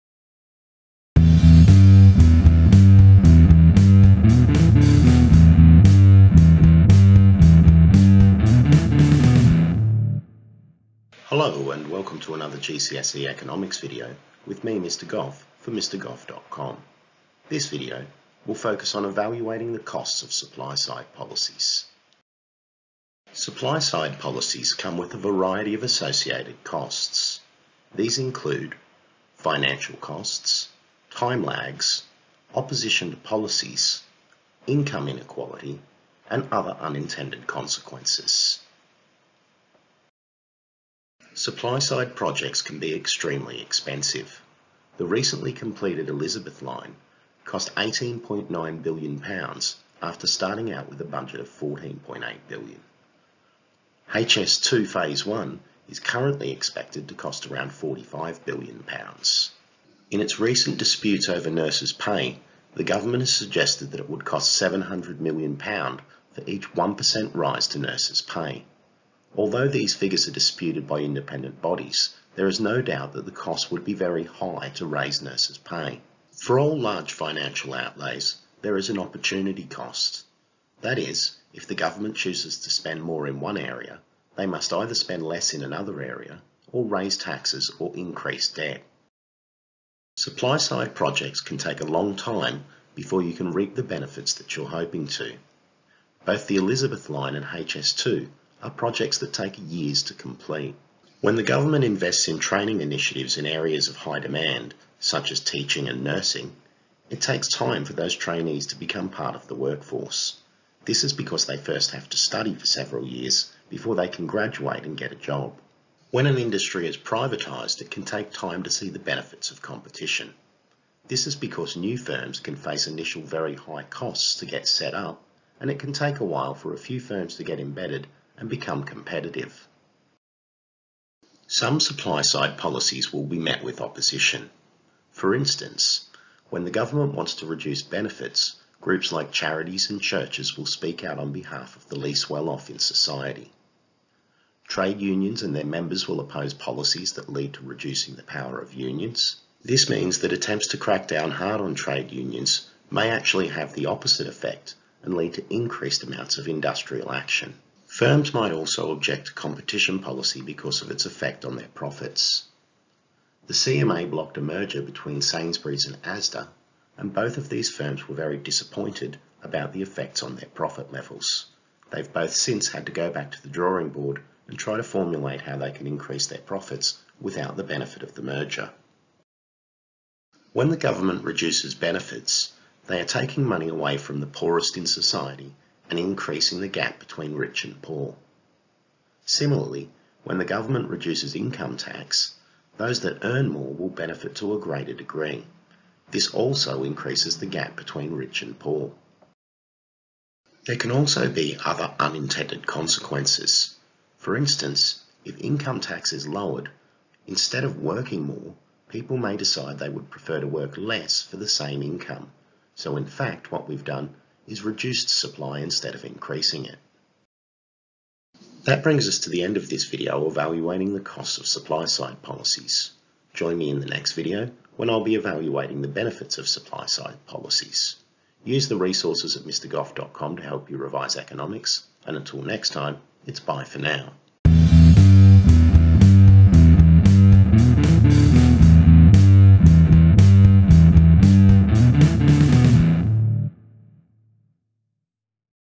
GCSE Economics: Evaluating the costs of supply side policies
A GCSE Economics video in a series based on Unit 2 of the OCR specification presented by Mr Goff from MrGoff.com. This video focuses on evaluating the costs of supply side policies.